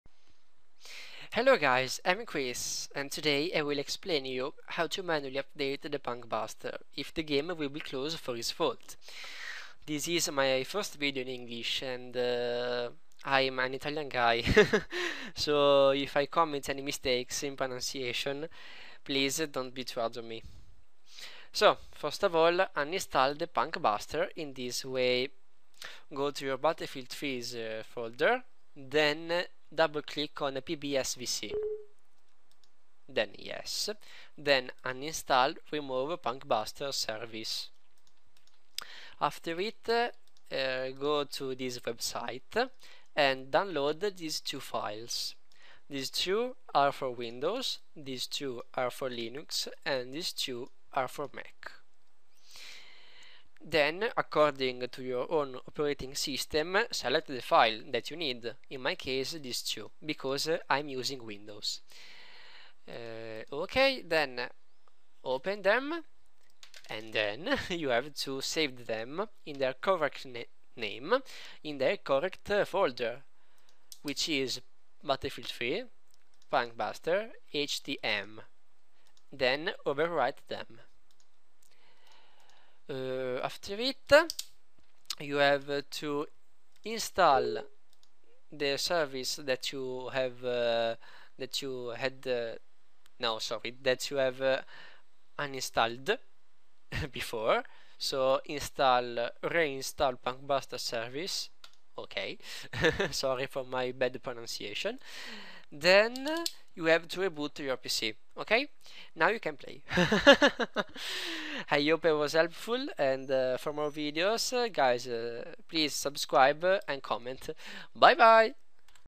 BF3 - How to update PunkBuster - Crash PnkBstr - Cr1sS_iT
How to update PunkBuster in case of crash